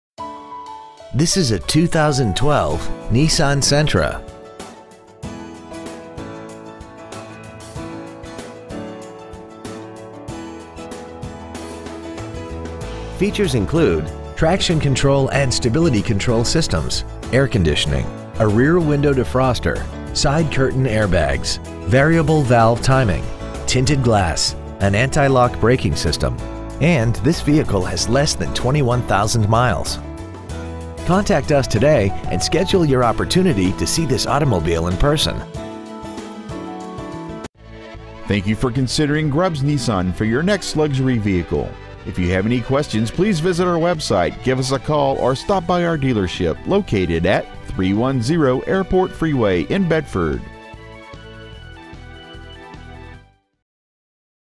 2012 Nissan Sentra Ft.Worth TX
We have been honored to serve the Bedford TX area , we promise that your experience at our dealership will exceed your expectations ! Year : 2012 Make : Nissan Model : Sentra Engine : 2.0L 4 cyls Exterior : White Miles : 20,938 Stock : N9239A Grubbs Nissan Airport Freeway Bedford , TX 76022